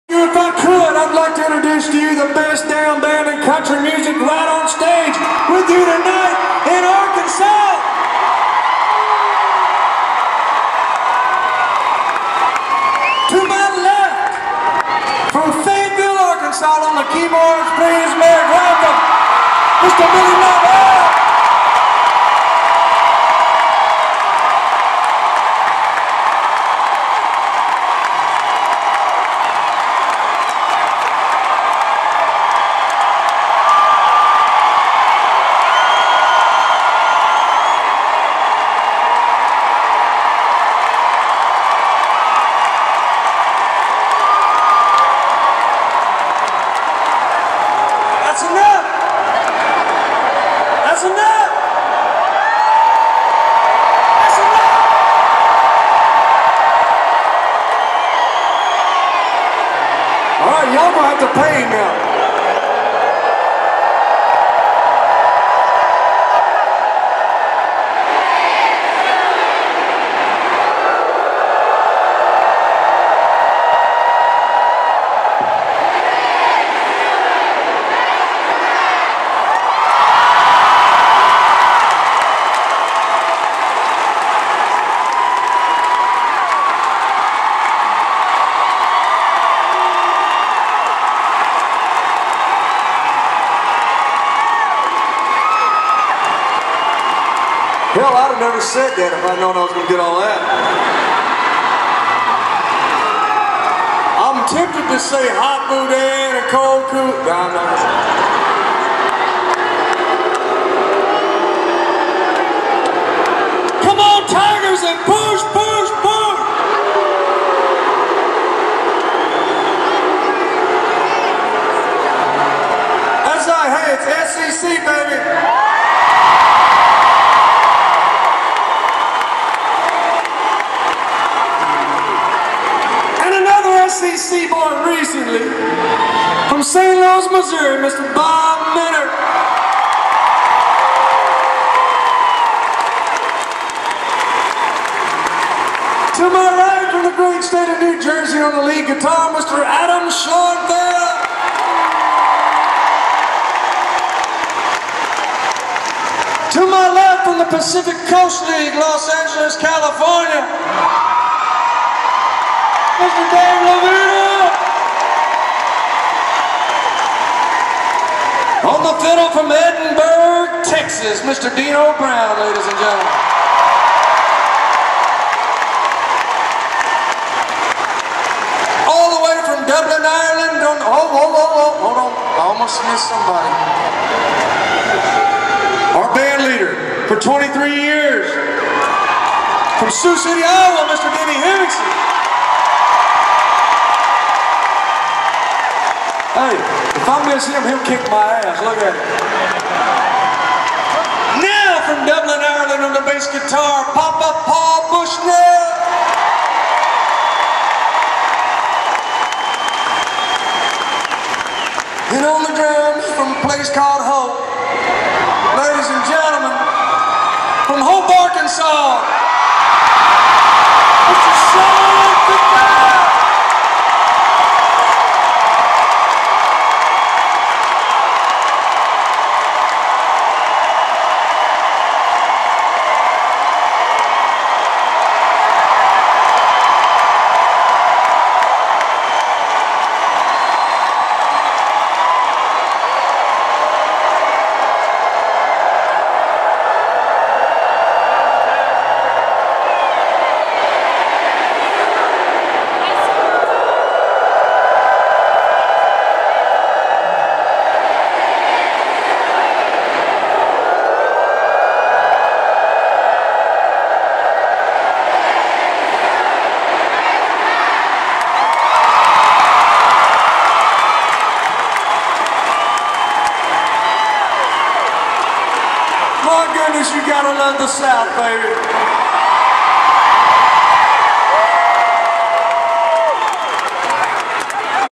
Wild Band Intro At Tim McGraw Concert at the Amp In Rogers Arkansas
The Sundown Heaven Town Tour Tim McGraw At The Amp Rogers Arkansas 8/7/14